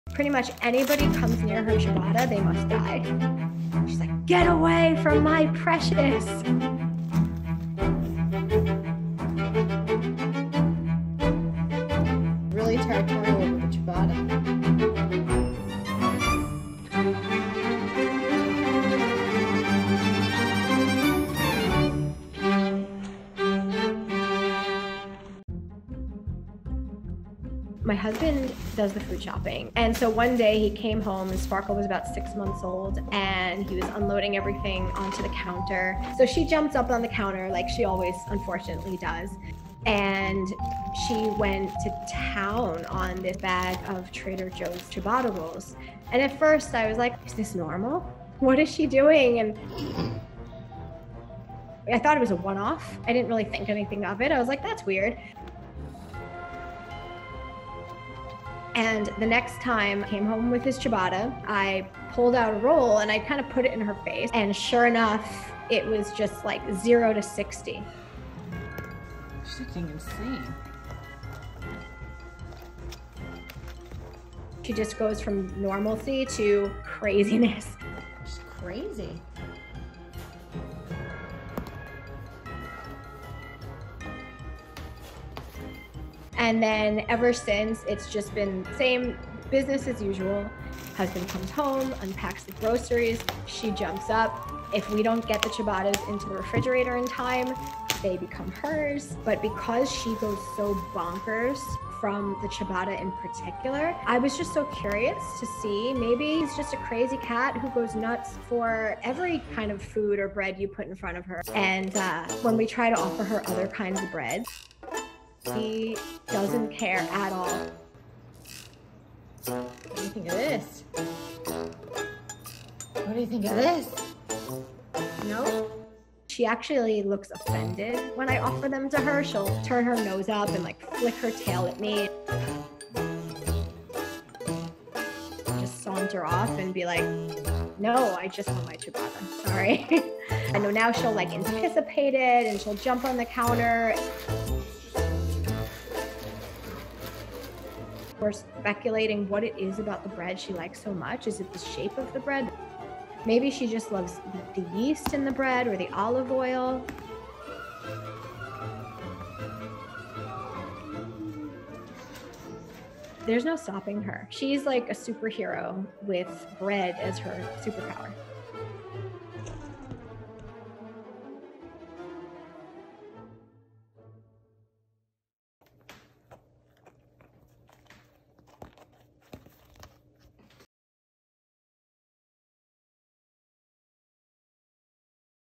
Cat goes WILD for Trader Joe's ciabatta! | The Dodo Cat Crazy
Cat is obsessed with Trader Joe's ciabatta